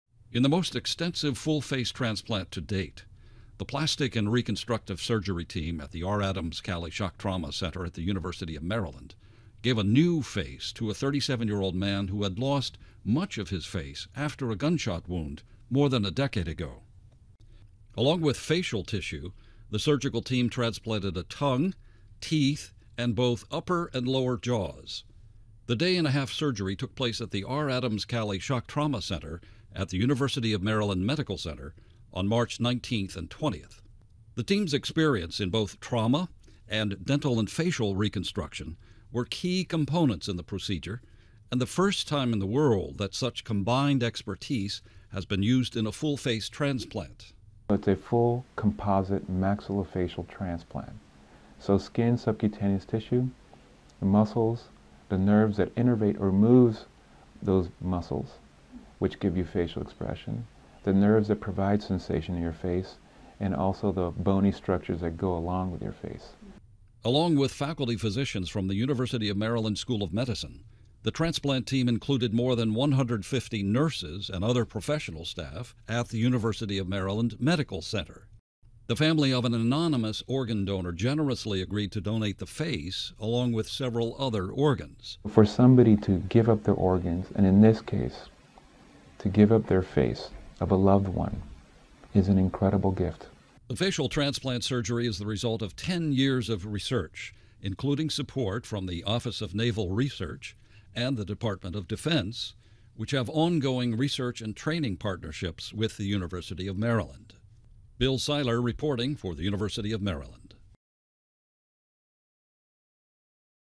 Full Face Transplant a Success
Surgeons at the University of Maryland succeed in the fullest face transplant ever.